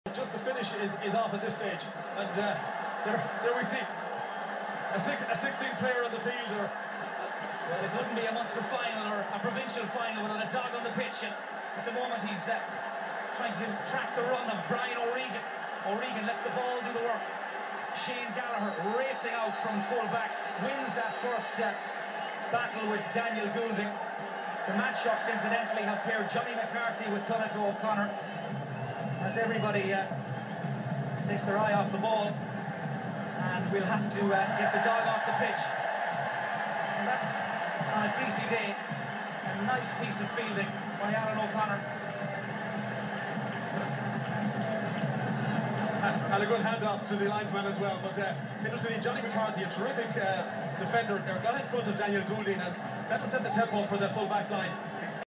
Dog on the pitch at the 2009 Munster Football Final!
A Dog decides to invade the pitch during the Munster Senior Football final between Cork and Limerick at Pairc Uí Caoimh. Nice tackle to stop him!